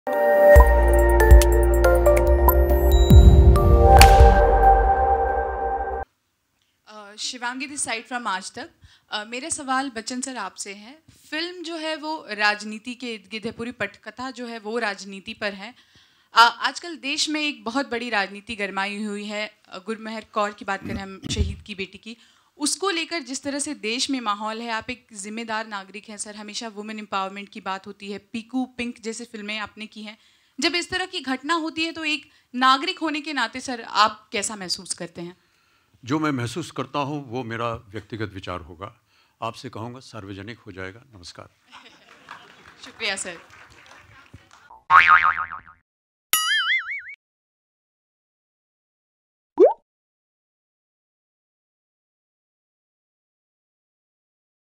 Amitabh Bachchan Best Reaction On Gurmehar kaur Controversy | Viralbollywood
Watch Amitabh Bachchan Reaction On Gurmehar kaur Troll In Social Media | Viralbollywood